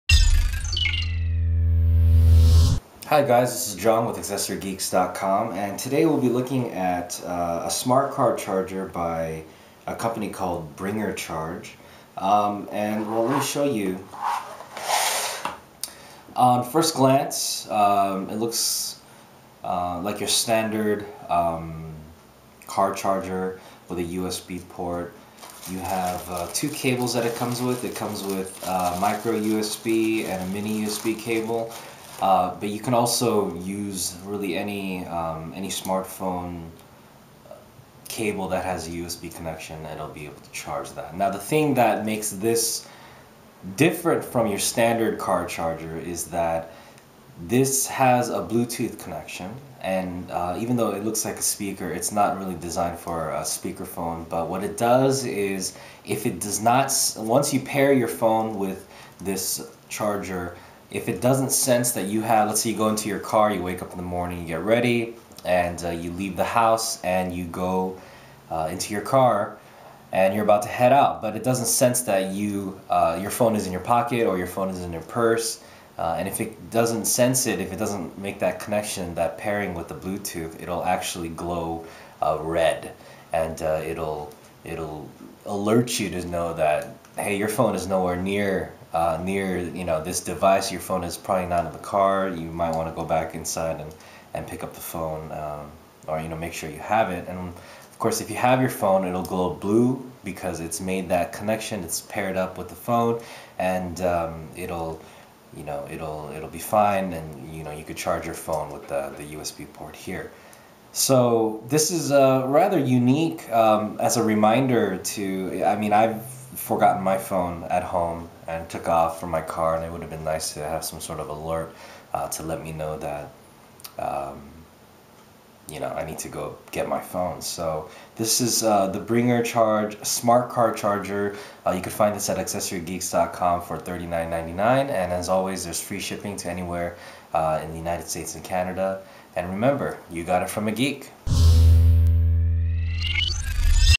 Bringrr Smart Car Cell Phone Charger
This unique Smart Car Charger could be the mobile charging solution you've been looking for!